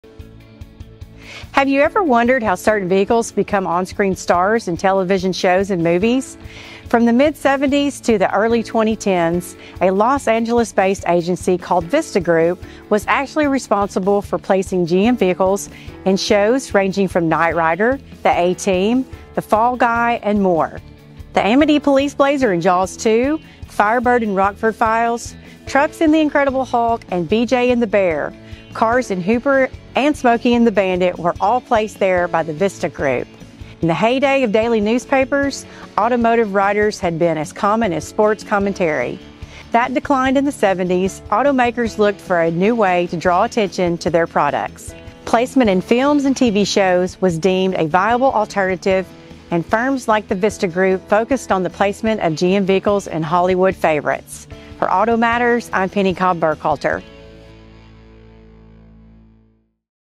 Auto Matters - The Vista Group
- Ever wonder how certain vehicles ended up in movies and television shows?  A firm called "The Vista Group" put them there!